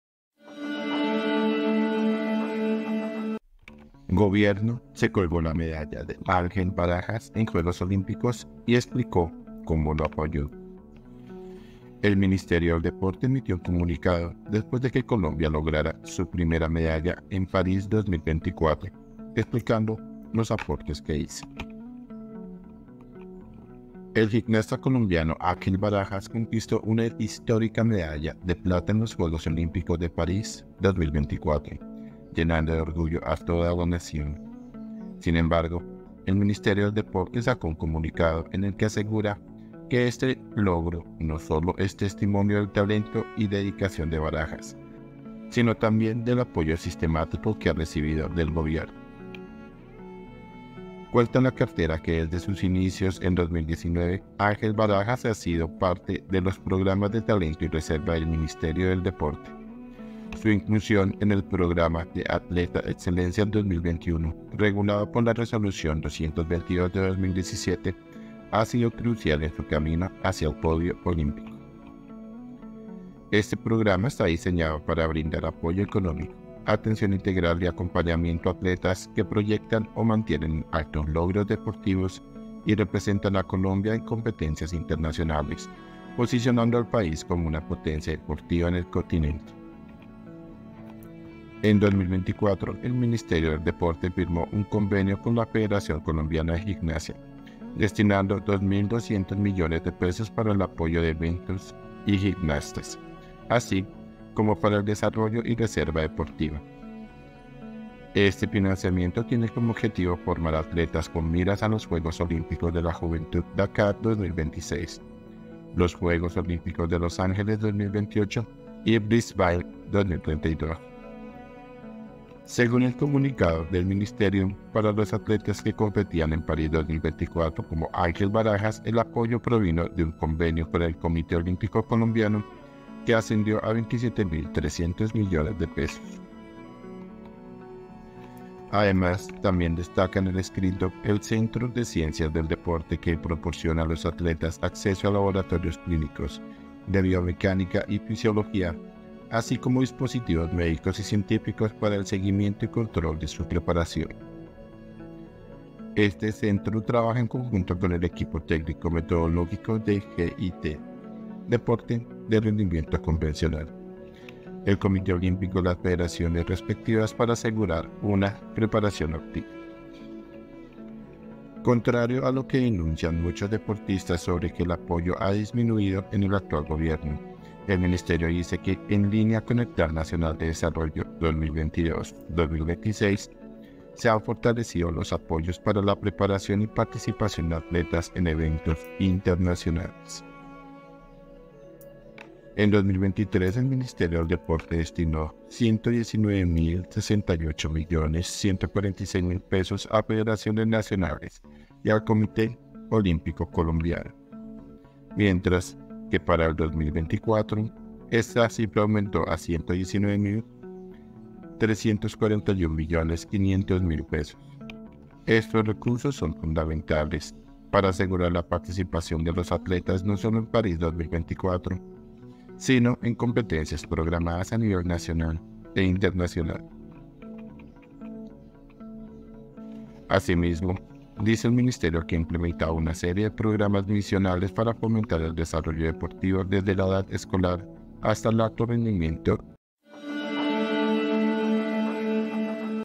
🔴 GOBIERNO SE COLGÓ LA MEDALLA DE ÁNGEL BARAJAS EN JUEGOS OLÍMPICOS Y EXPLICÓ CÓMO LO APOYÓ
El Ministerio del Deporte emitió un comunicado después de que Colombia lograra su primera medalla en París 2024 explicando los aportes que hizo.
EL COLOMBIANO